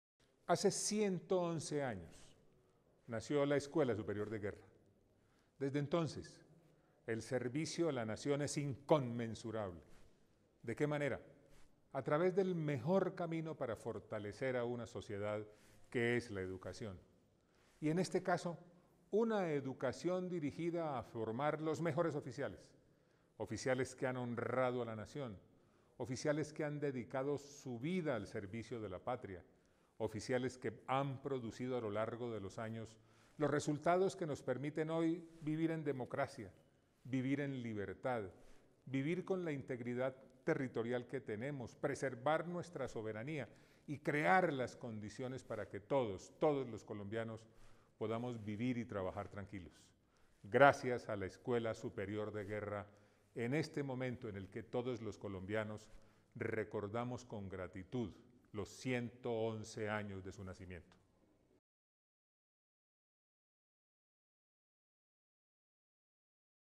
ESDEGUE: 111 Años al servicio de Colombia
Hace 111 años se creó la Escuela Superior de Guerra con la misión de fortalecer la capacitación de nuestra Fuerza Pública.

Hoy, honramos un nuevo aniversario de una de las mejores instituciones educativas del hemisferio.